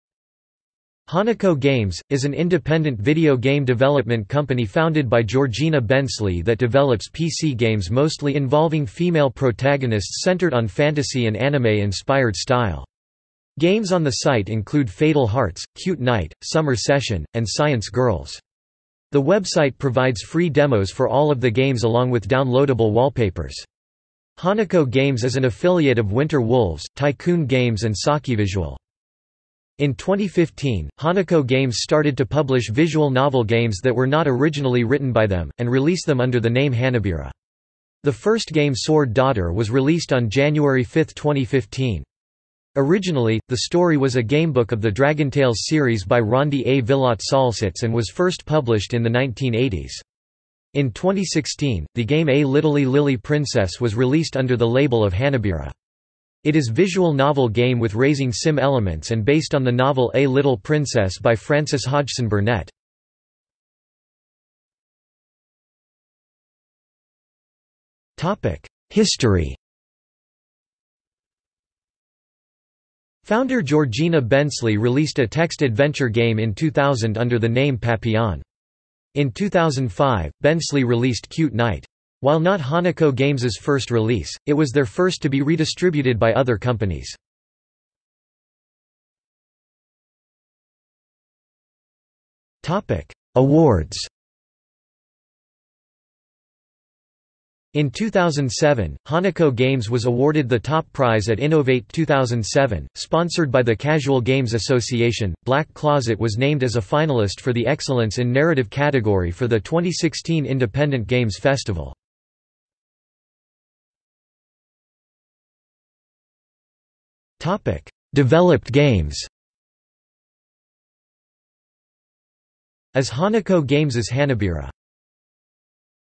Hanako Games | Wikipedia audio article
00:01:04 1 History
00:01:29 2 Awards
00:01:53 3 Developed games

- Socrates

SUMMARY
Hanako Games, is an independent video game development company founded by Georgina Bensley that develops PC games mostly involving female protagonists centered on fantasy- and anime-inspired style. Games on the site include Fatal Hearts, Cute Knight, Summer Session, and Science Girls. The website provides free demos for all of the games along with downloadable wallpapers.
In 2015, Hanako Games started to publish visual novel games that were not originally written by them, and release them under the name Hanabira. The first game Sword Daughter was released on January 5, 2015. Originally, the story was a gamebook of the Dragontales series by Rhondi A. Vilott Salsitz and was first published in the 1980s. In 2016, the game A Littly Lily Princess was released under the label of Hanabira. It is visual novel game with raising sim elements and based on the novel A Little Princess by Frances Hodgson Burnett.